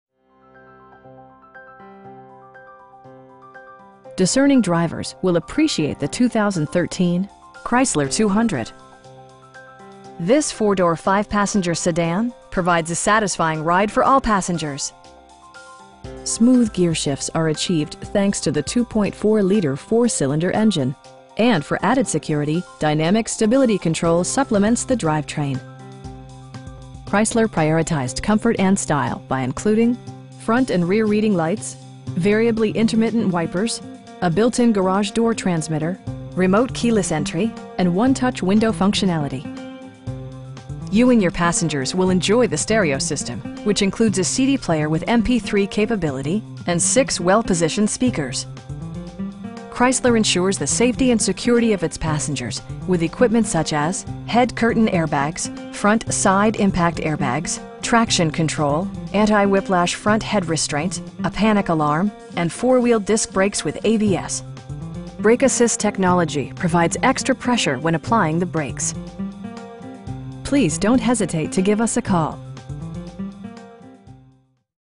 2013 Chrysler 200 Touring in Louisville, KY 40207
Discerning drivers will appreciate the 2013 Chrysler 200. This 4 door, 5 passenger sedan provides a satisfying ride for all passengers. Smooth gearshifts are achieved thanks to the 2.4 liter 4 cylinder engine, and for added security, dynamic Stability Control supplements the drivetrain. Chrysler prioritized comfort and style by including: front and rear reading lights, variably intermittent wipers, a built-in garage door transmitter, front bucket seats, tilt and telescoping steering wheel, remote keyless entry, and 1-touch window functionality. You and your passengers will enjoy the stereo system, which includes a CD player with MP3 capability, and 6 well positioned speakers. Chrysler ensures the safety and security of its passengers with equipment such as: head curtain airbags, front SIDE impact airbags, traction control, anti-whiplash front head restraints, a panic alarm, and 4 wheel disc brakes with ABS. Brake assist technology provides extra pressure when applying the brakes. We pride ourselves on providing excellent customer service. Please don't hesitate to give us a call.